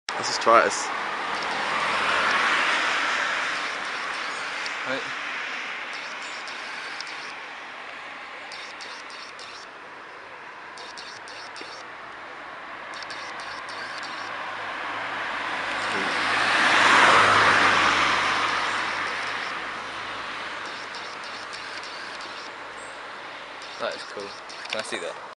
Nikon D3100 Auto Focus Test - Skateboard
** Watch in full 1080p HD **

This is a quick auto focus test done on my new nikon D3100 with the kit lens.